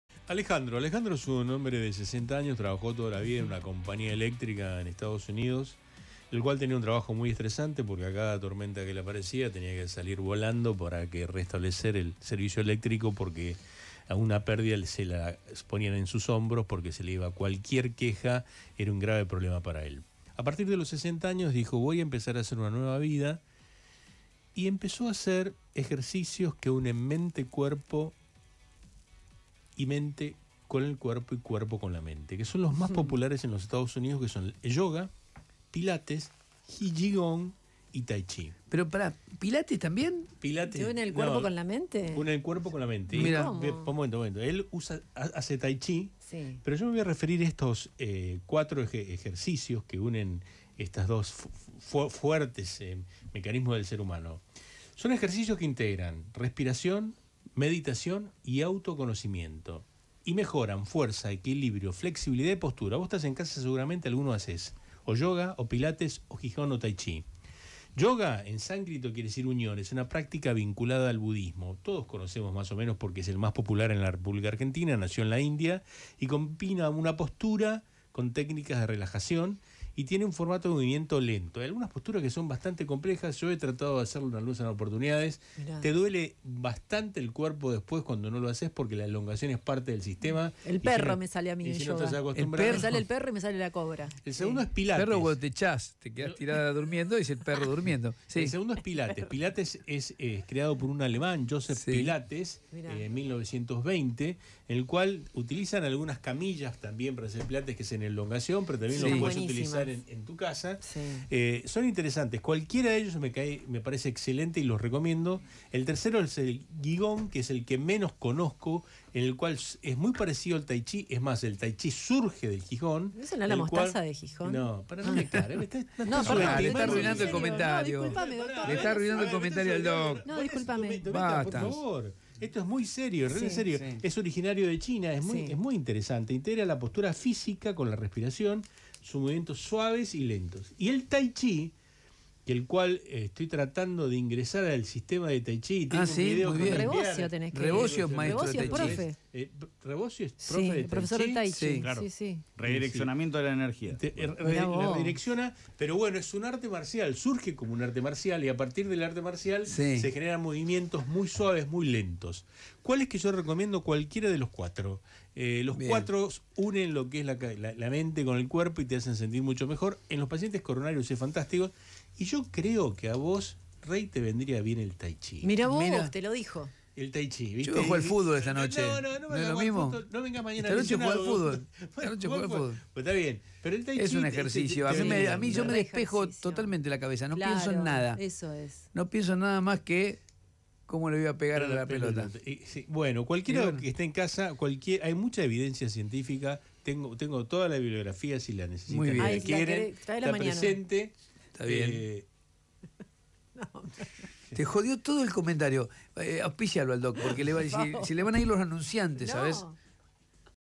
Yoga - Pilates - QiGong - Taichi ¿Cuál es el mejor para vos? | Dr. Jorge Tartaglione | Radio Con Vos
En esta columna te hablo sobre qué actividad es la mejor para vos y para tu salud.